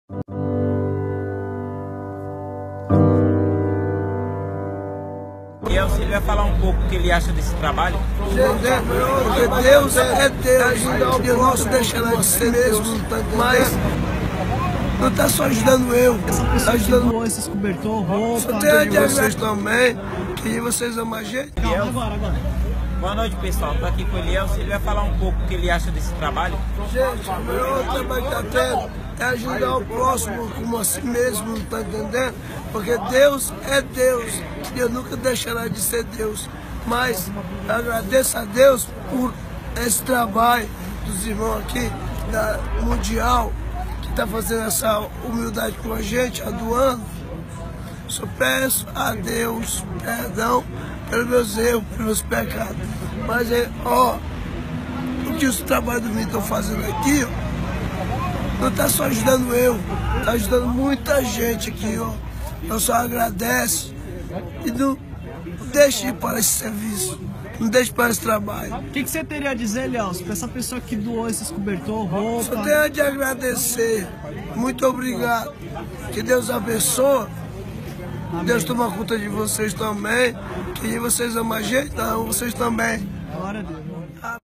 VEJA O QUE O EVAGELISMO FAZ | TRABALHO DE EVANGELISMO NAS RUAS - José Junior
Evangelismo na rua, evangelizamos muitos moradores de rua, o evangelismo tem alcançado muitas vidas.